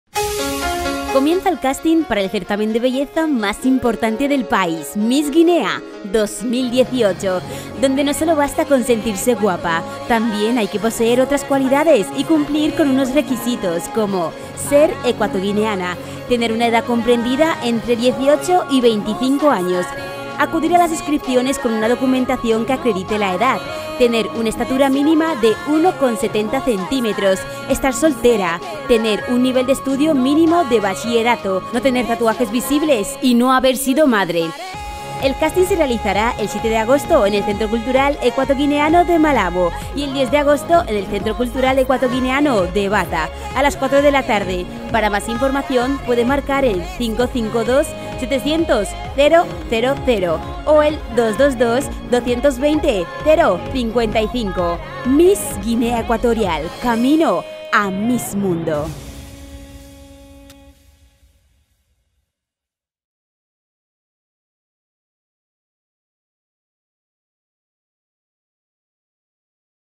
CASTING MISS GUINEA ECUATORIAL 2018
Mis Guinea Ecuatorial
Fecha del Casting:
7 De Agosto Malabo
10 De Agosto Bata

Lugar: Centro Cultural Ecuatoguineano (Bata Y Malabo)
Hora: 16 Horas

Requisitos:
1. Nacionalidad Ecuatoguineana
2. Edad De 18 A 25 Años
3. Documentación Que Acredite La Edad
4. Soltera
5. Estatura 1,70 Cm
6. Estudios Mínimos De Bachillerato
7. Sin Tatuajes Visibles
8. No haber sido madre

Miss Guinea Ecuatorial, Camino A Miss Mundo